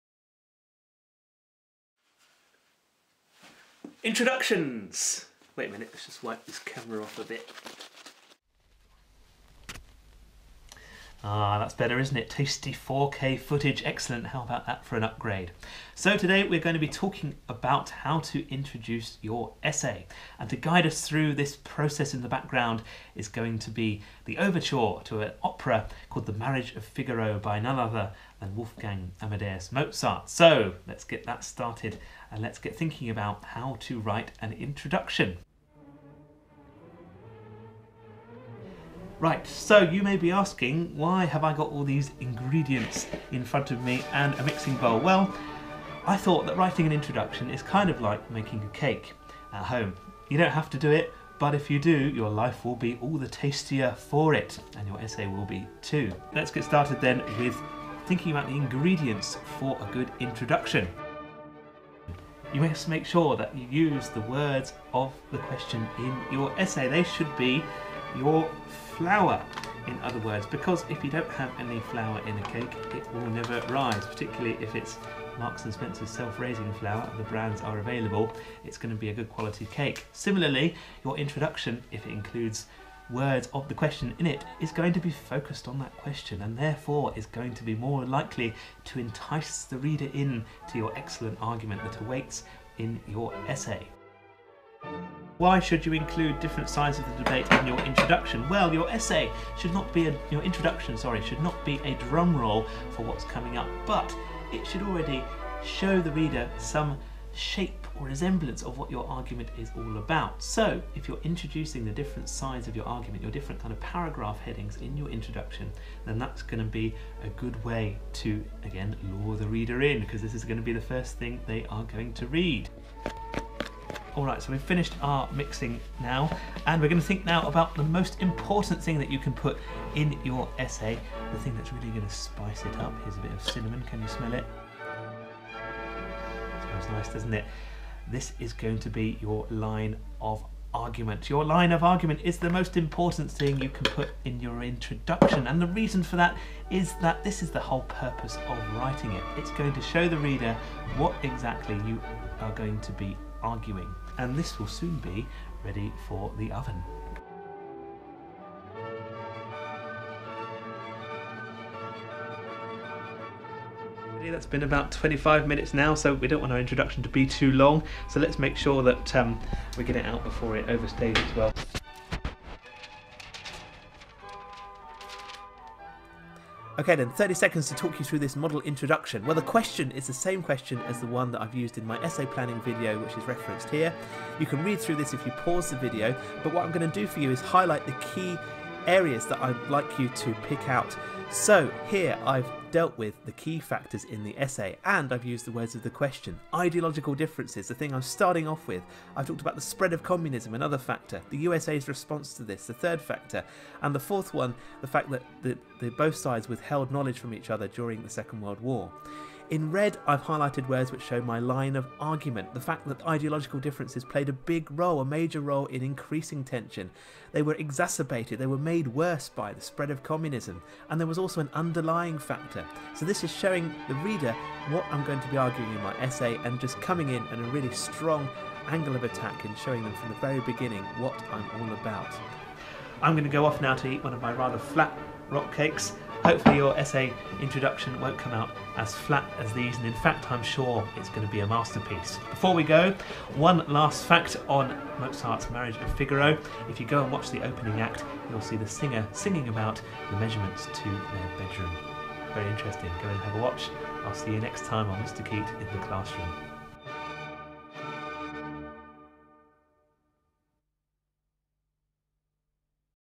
How to write an introduction
A quick guide on how to write an introduction for a History essay.